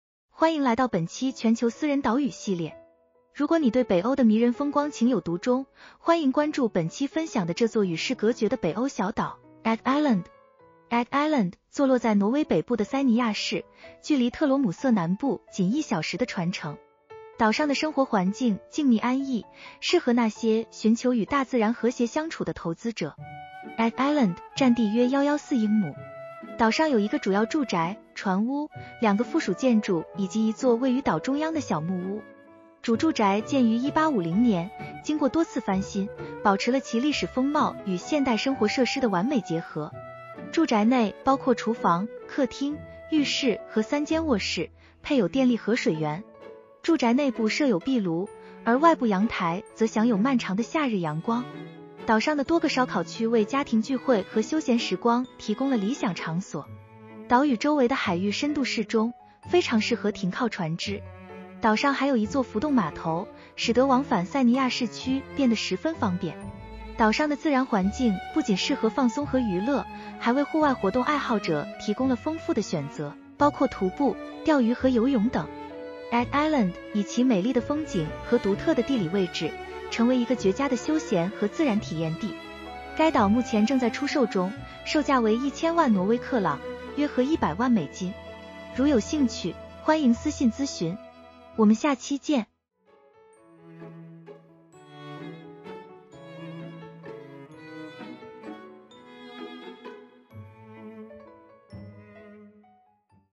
0107 Eggøya｜Private Island for Sale in Norway
If you are enchanted by the charming landscapes of Northern Europe, welcome to explore Eggøya, an isolated Nordic island featured in this issue. If interested, welcome to send your inquiry.